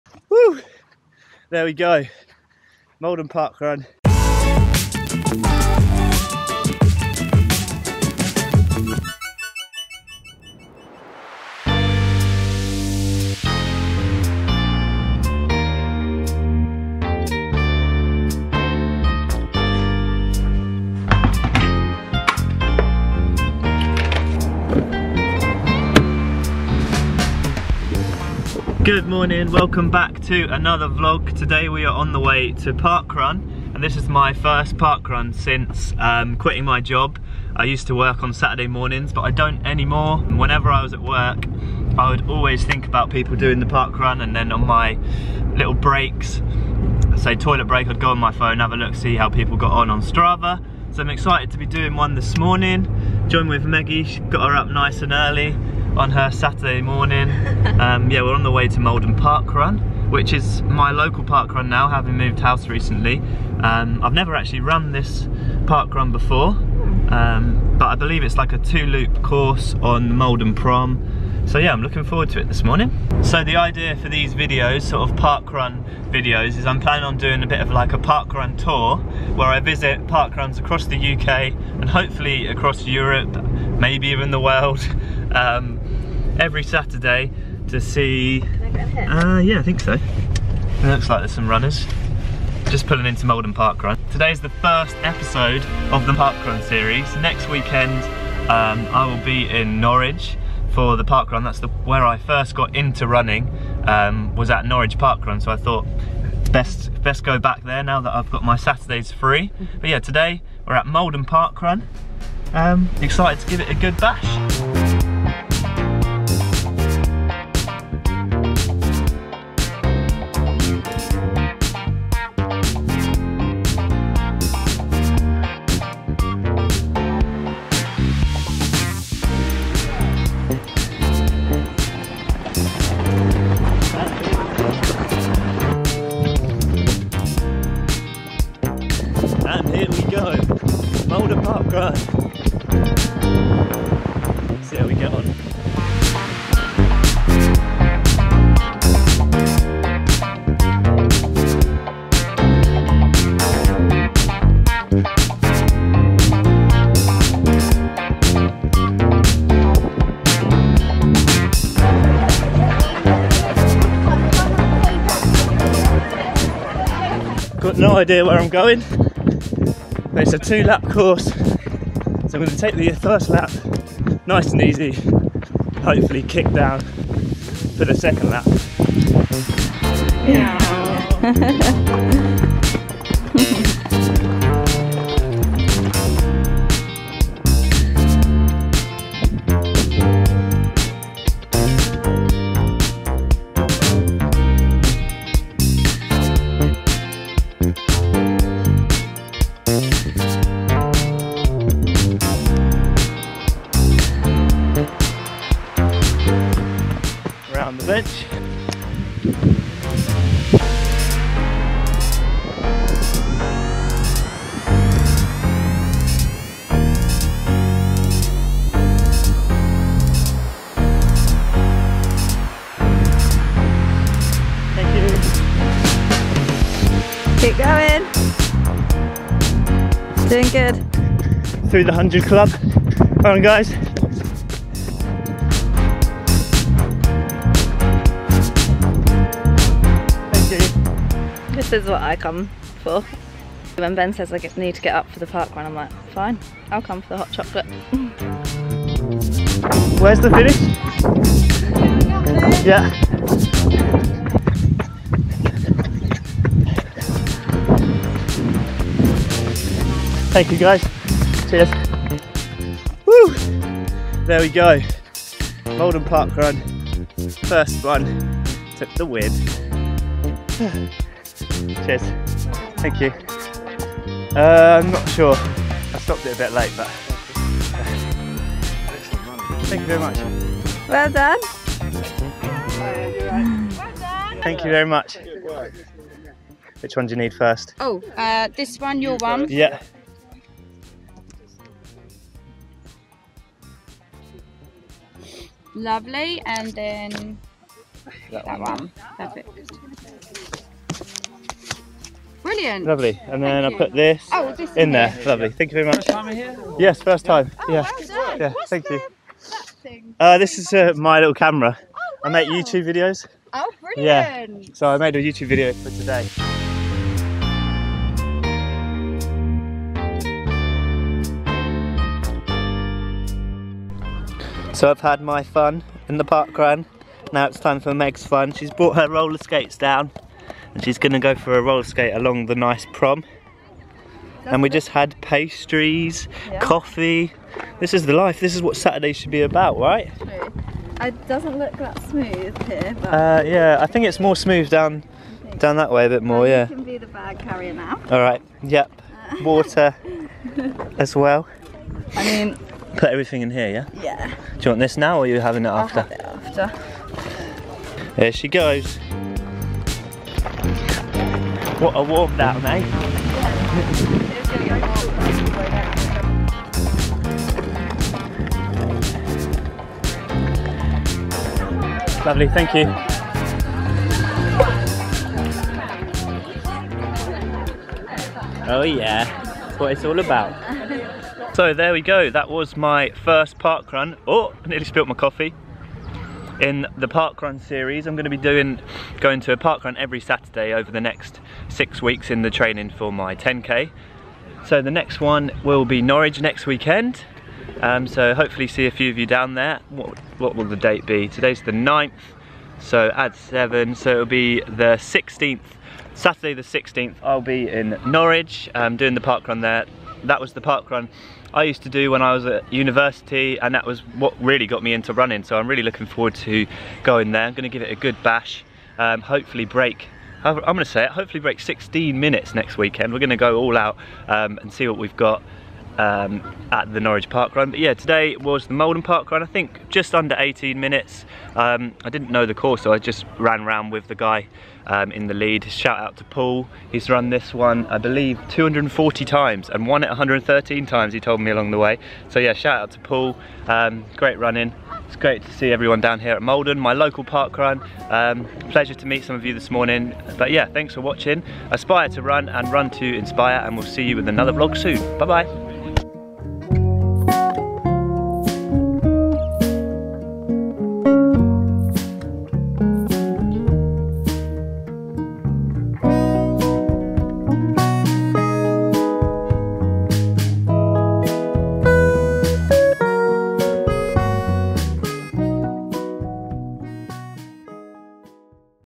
RUNNING A NEW 5K PARKRUN EVERY WEEK (EPISODE 1)
EPISODE 1. Today i ran my local parkrun in Maldon, Essex now that i finally have my Saturday mornings back! I've missed these so much due to work commitments so have decided to run a new one every week. Next stop: Norwich (eaton park) 16th October. Comment bellow where i should go next!!!

ASPIRE TO RUN, RUN TO INSPIRE